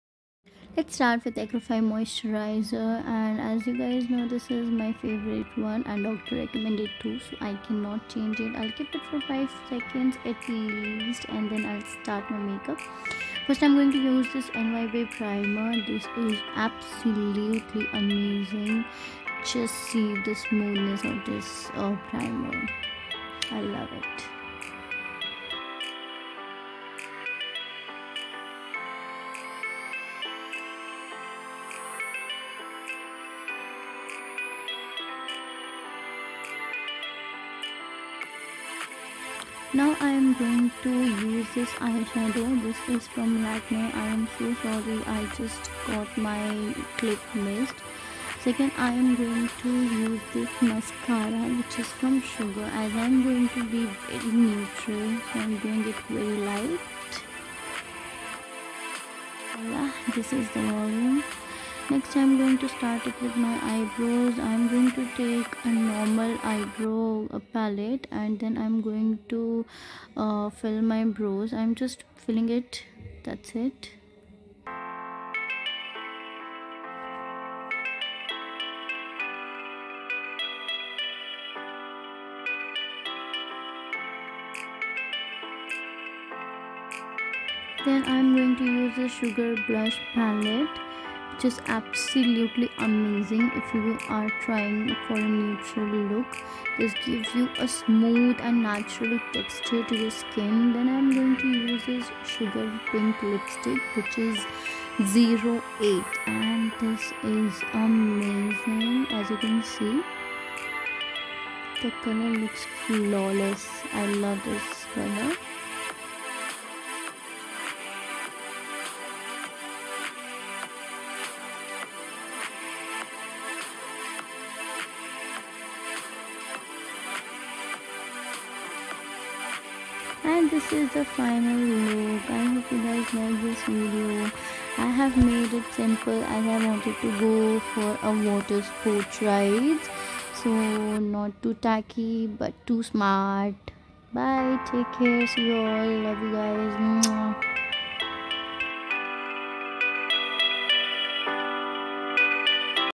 Goa makeup look | GRWM FOR GOA BEACH | NO foundation makeup look .
I usually do this makeup for water sports or for sunny day.